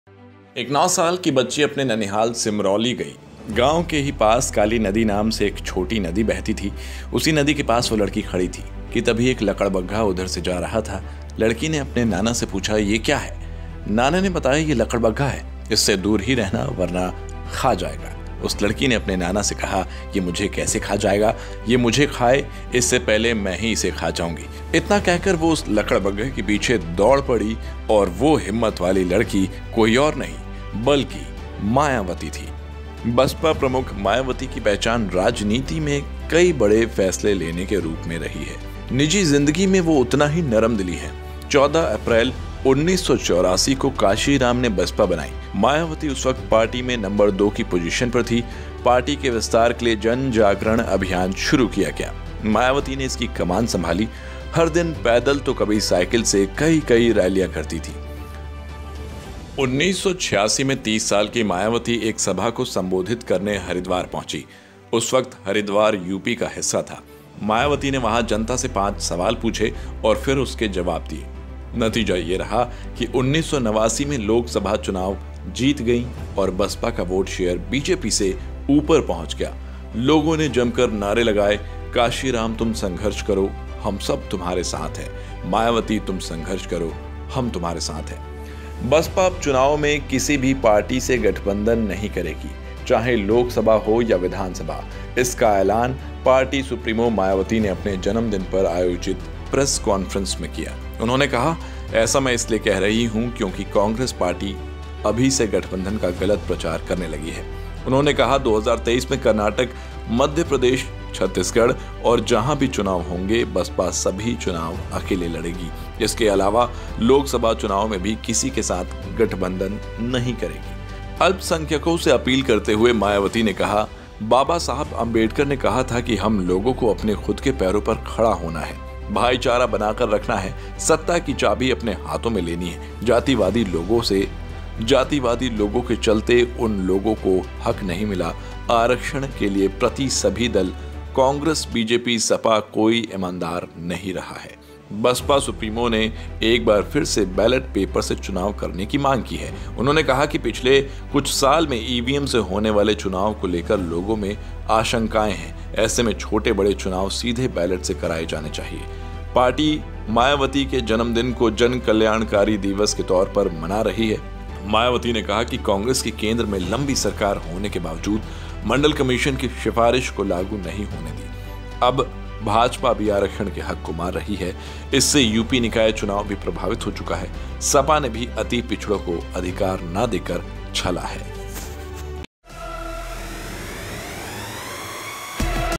2024 को लेकर Mayawati ने कर दिया खुलासा, इस तरीके से लड़ेगीं चुनाव
2024 को लेकर Mayawati ने कर दिया खुलासा, इस तरीके से लड़ेगीं चुनाव